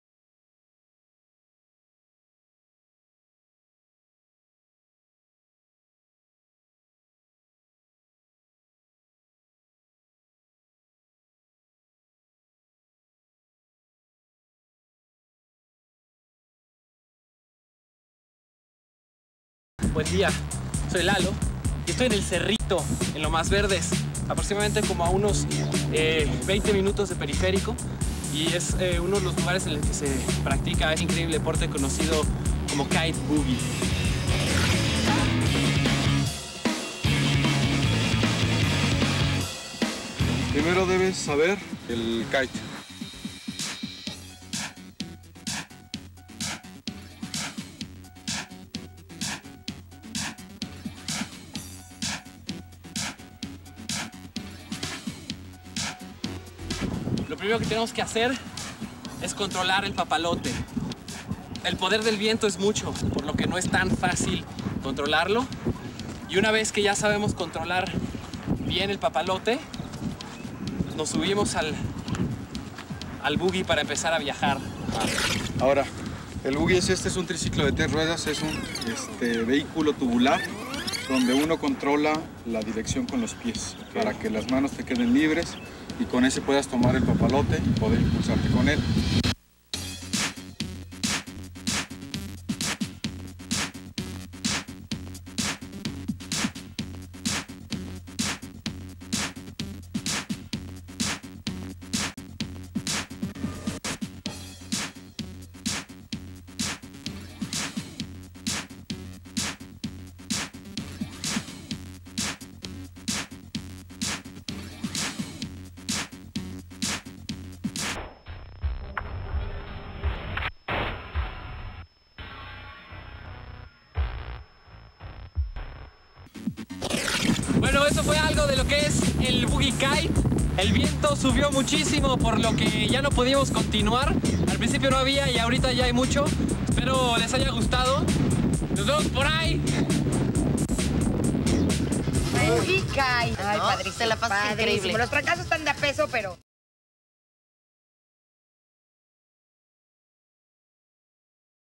Kitebuggy en el Cerrito por TV Azteca
Reportaje realizado para TV Azteca de KiteBuggy en lo que era el Cerrito que fue un lugar apacible y tranquilo, ahora deshecho por la construccion de un WalMart.